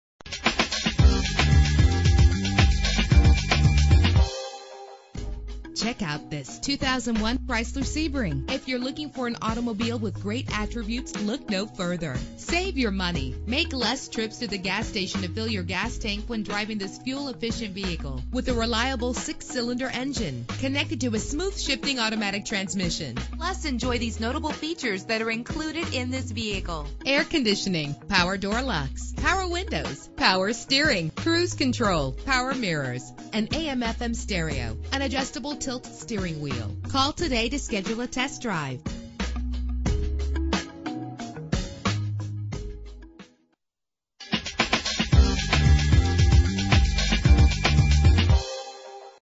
2001 Chrysler Sebring - Antioch IL
Year: 2001
Make: Chrysler
Model: Sebring
Trim: LX
Engine: 2.7 liter 6 cylinder 24 valve
Transmission: 4-Speed Automatic
Color: Silver
Mileage: 137040
Address:
118 Rt 173